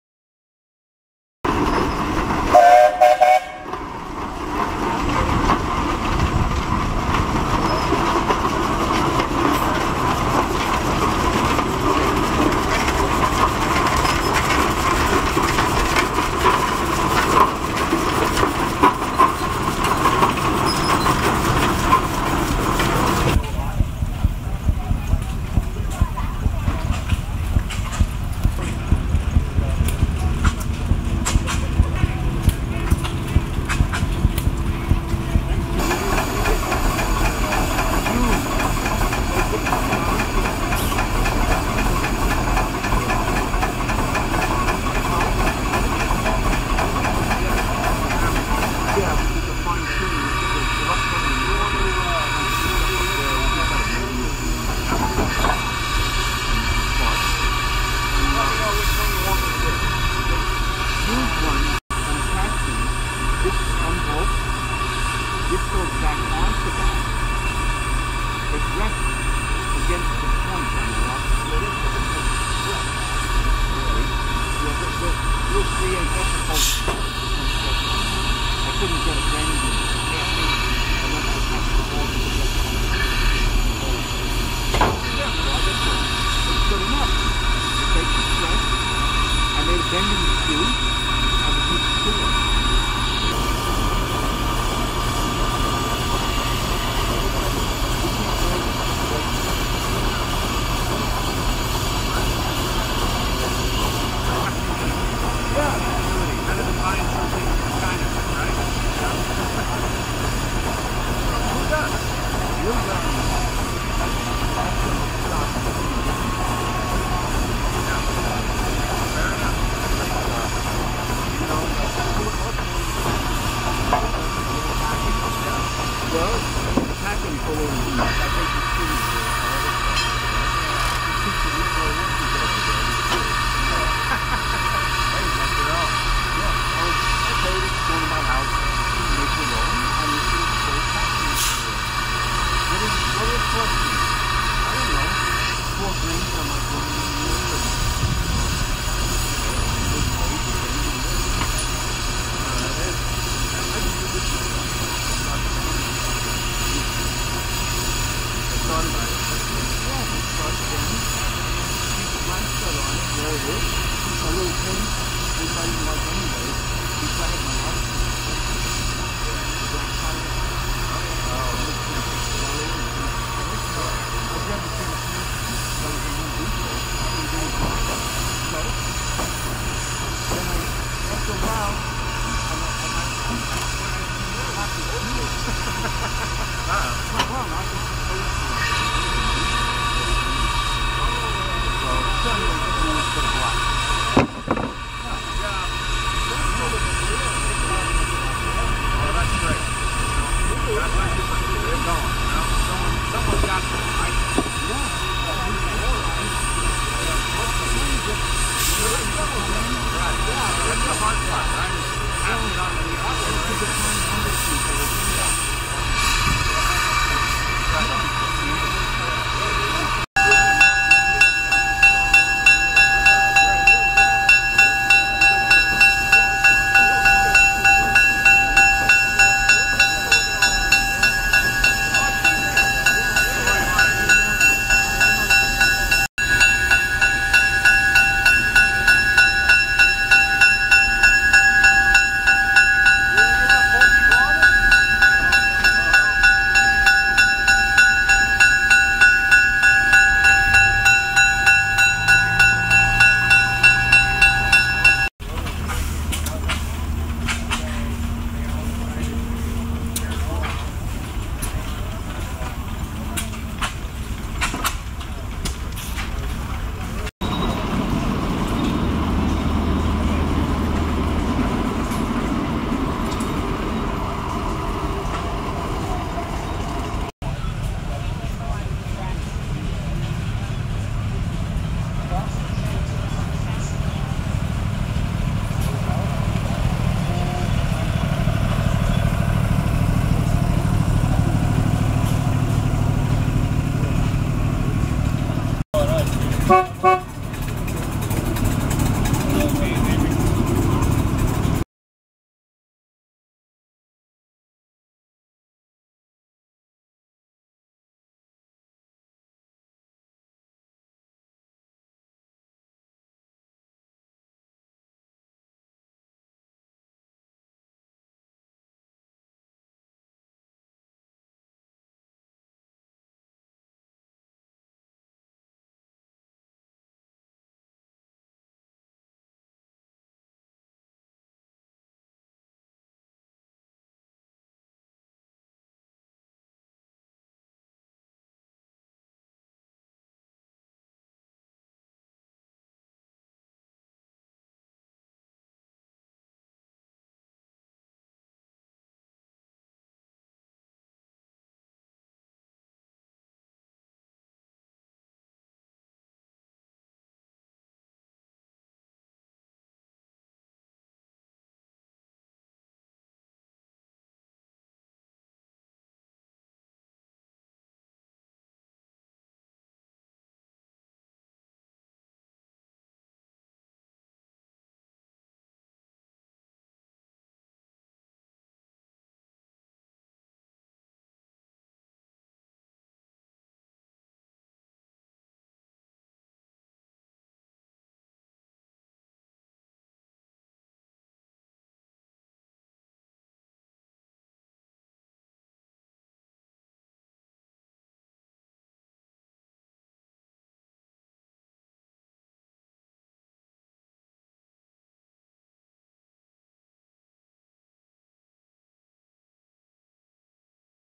The Great Oregon Steam-Up 2023
If you like old equipment and especially steam powered machinery, this is the place to be the last weekend in July and the first weekend in August. Put on by volunteers with a love for the past, it is fun for young and old. A special treat was watching my friend flag the  the steam powered 1928 Bucyrus Erie crane on rails. As you watch the crane operate, look for the operator or leverman.  His position is hidden back in the machinery unlike today's crane operators with big glass cabs and often cameras and screens to aid them.